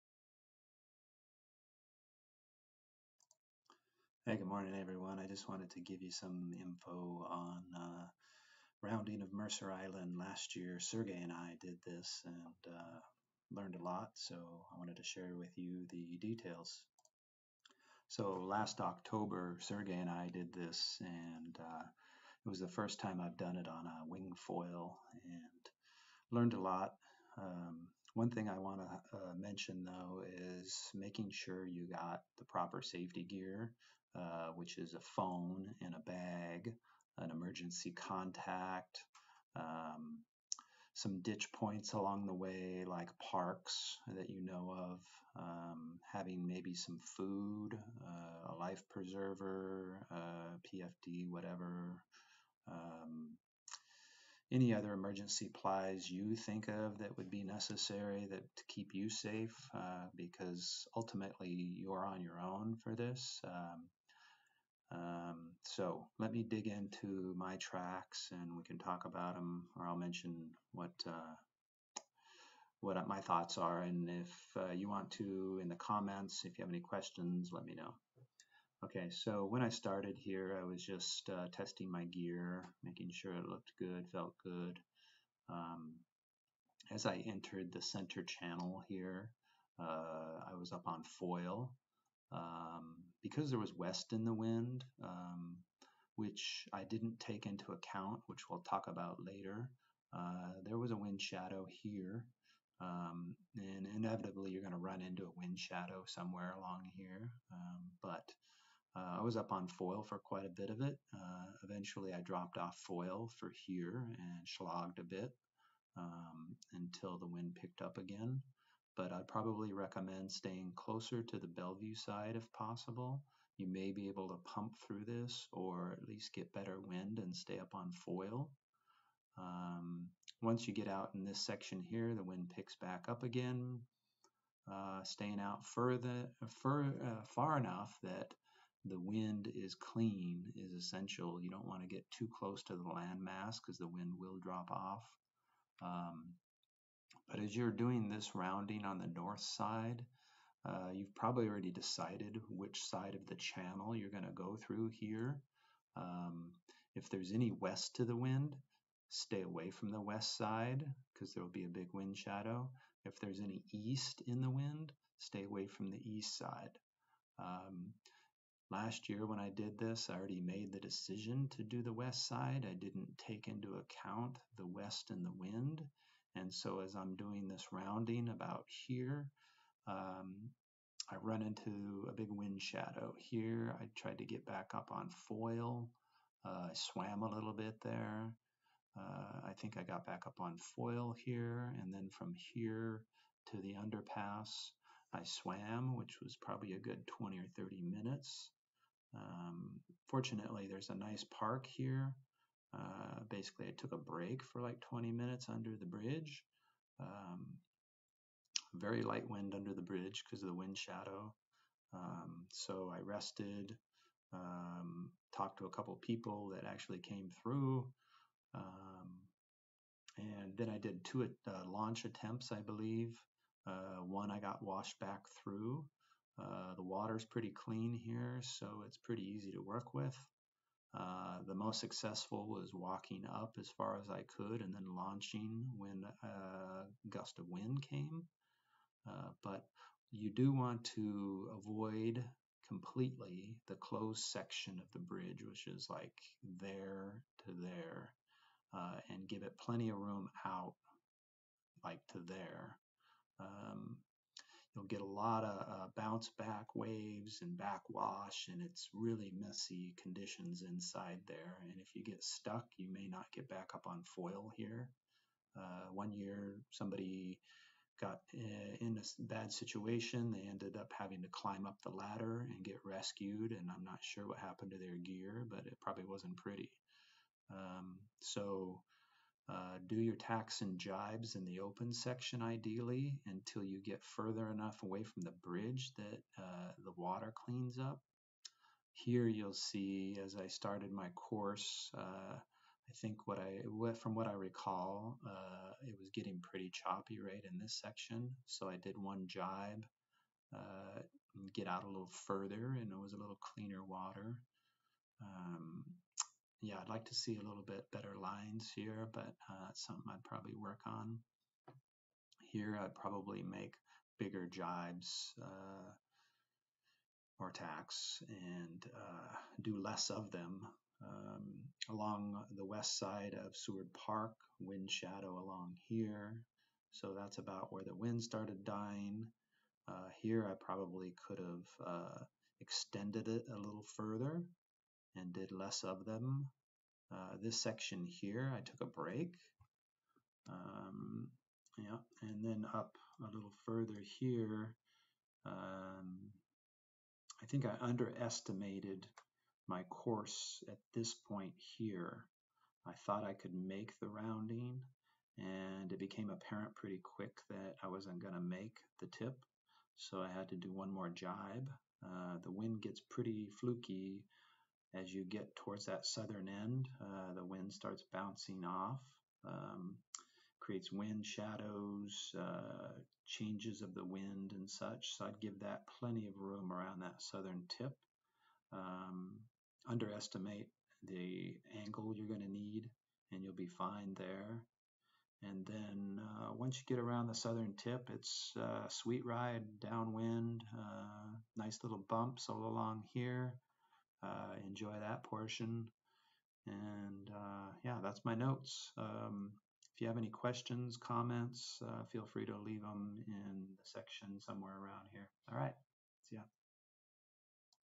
Winduro2020 Wingfoiling
First Wing to complete the Winduro which is a 20+ year event in Seattle. Winduro is a test of endurance for windfoils, windsurfers, and now wingfoilers.